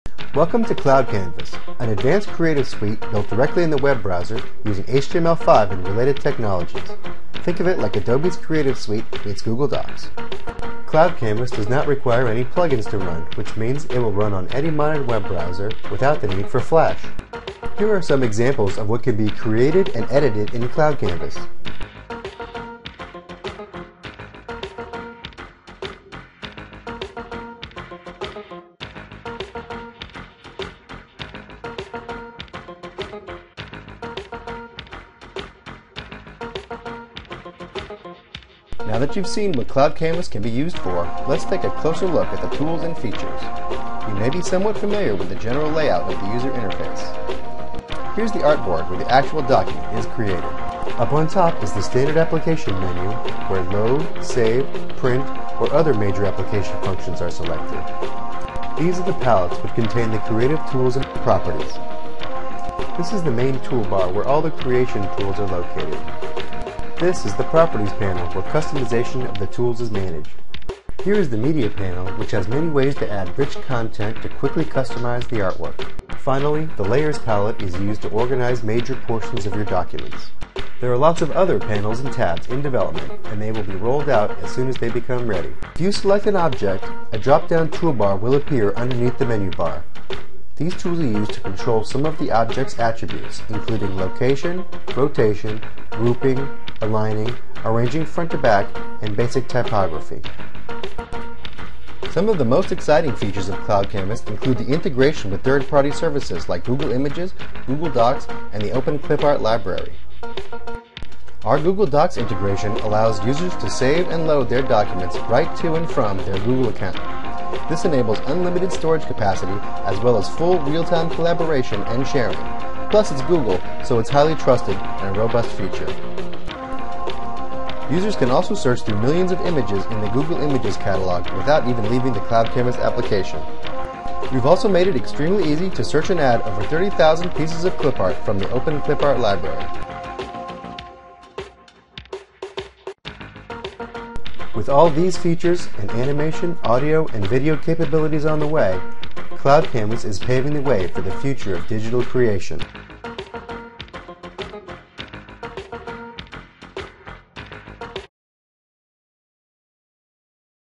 CloudCanvas Launch Demo Video
This is a brief overview of the CloudCanvas online Creative Suite.